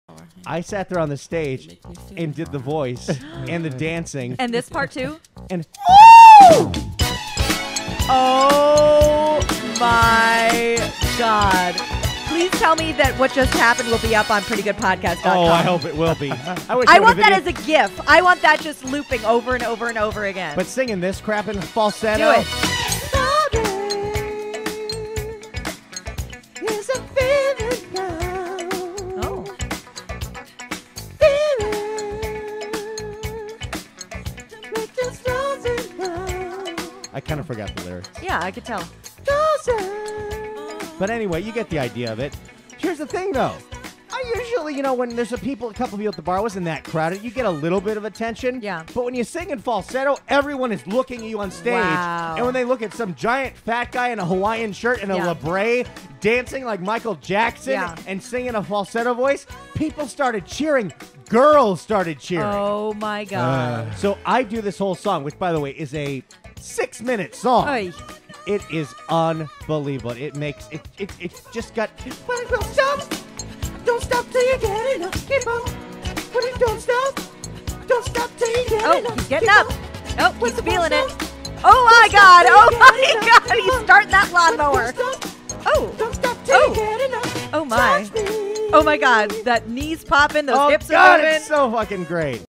PGP Clip of the Day 9-28-11
Randy Wang Busts some of the moves he picked up from the Michael Jackson game on Kinect.